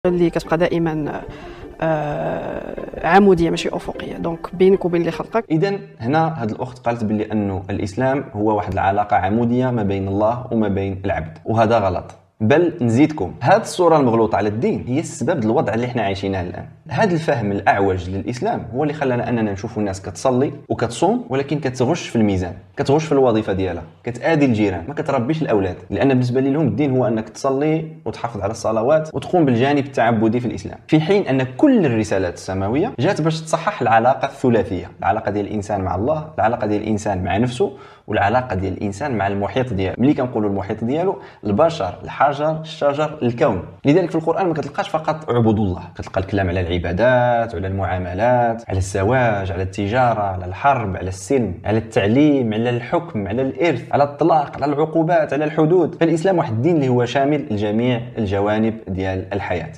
مقتطف من الحلقة 8
مقتطف من الحلقة 8 - سلسلة_رياكشن فيه رد على ادعاء "فاسيكي" بأن الدين هو علاقة عمودية